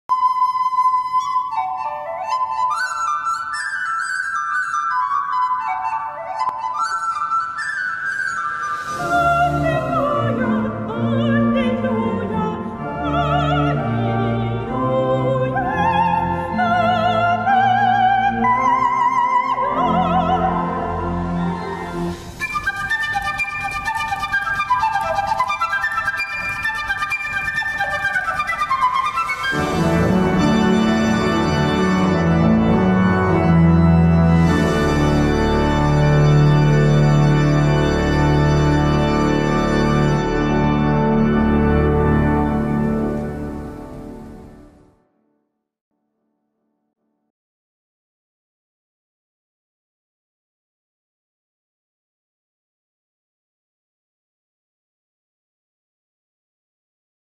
ШEДЕВРЫ КЛАССИКИ В СОБОРЕ. Орган, вокал сопрано и флейта звучат в стенах собора.
Невероятное трио  Екатерина Монисова (сопрано), Максим Рубцов (флейта) и Марина Омельченко (орган) исполняют классические произведения в стенах Римско-Католического кафедрального собора в Москве. Полная запись концерта в эфире канала Classic Music.